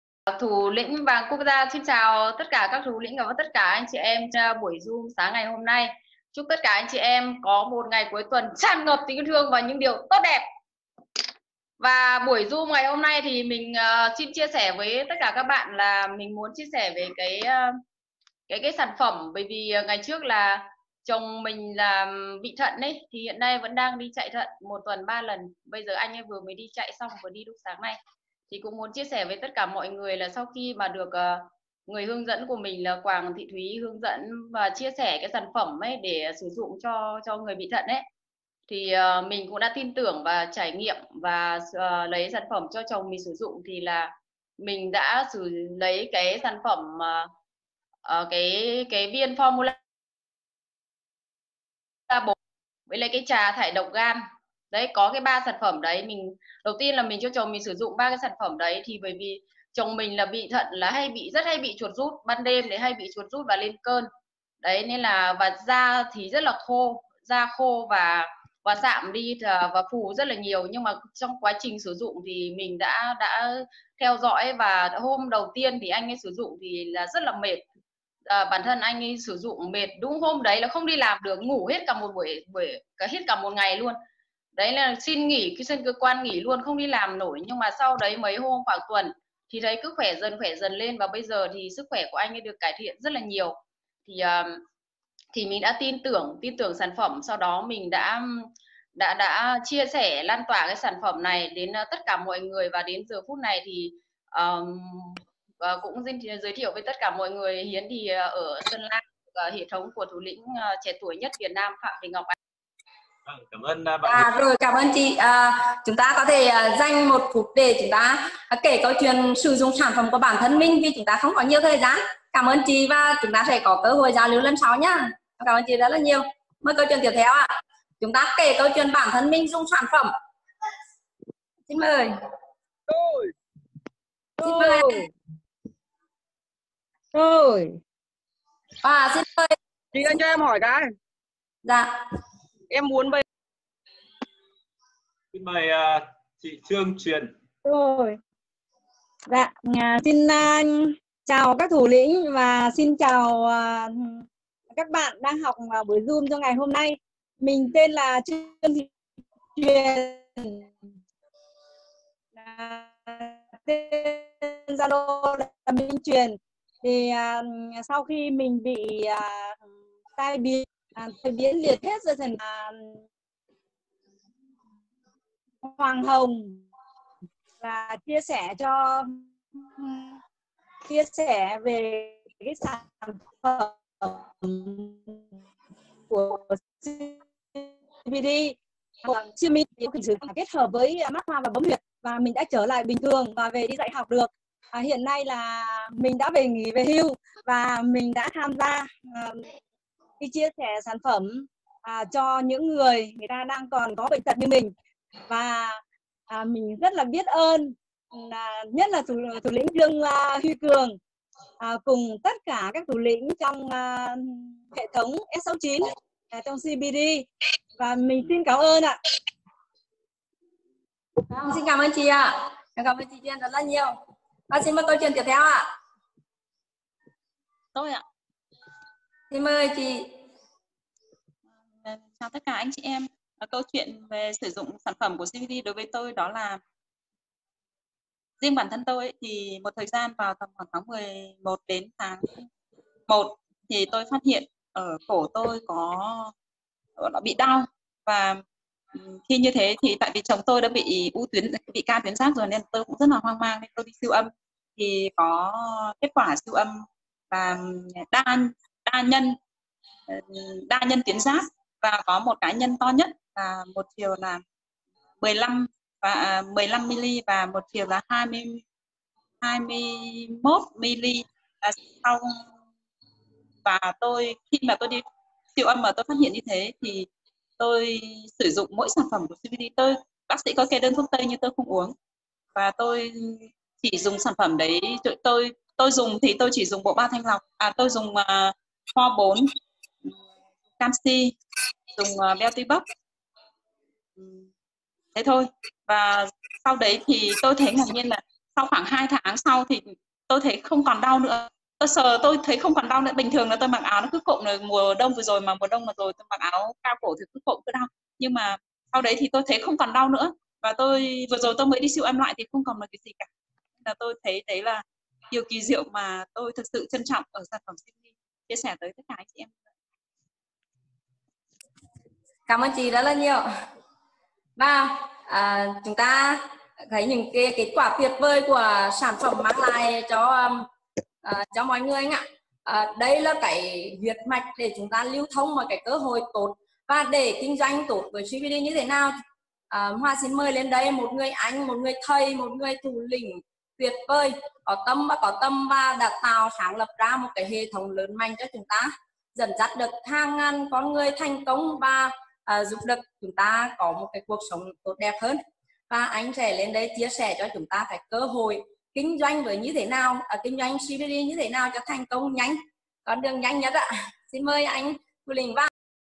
Người sau tai biến, đa nhân tuyến giáp phục hồi với Siberi
"Mình lấy Formula 4 và trà thải độc cho chồng mình đang chạy thận để dùng thêm. Chồng mình hay bị chuột rút vào ban đêm và lên cơn, da rất khô, sạm, phù rất nhiều. Giờ sức khỏe anh ấy được cải thiện rất nhiều"

"Sau khi bị tai biến, tôi dùng sản phẩm Siberi kết hợp massage và bấm huyệt nay sức khỏe tôi đã trở lại bình thường và đi dạy học được. Hiện nay tôi rất vui vì công việc chia sẻ sản phẩm đến những người khác."

"Tháng 11- tháng 1 tôi phát hiện cổ mình bị đau, kết quả siêu âm là đa nhân tuyến giáp (15 mm và 21 mm), sau 2 tháng dùng Siberi, tôi sờ vào thì không còn đau khi mặc áo cao cổ"

TỰ HÀO VÌ CÔNG VIỆC MÌNH LÀM QUÁ 💛💛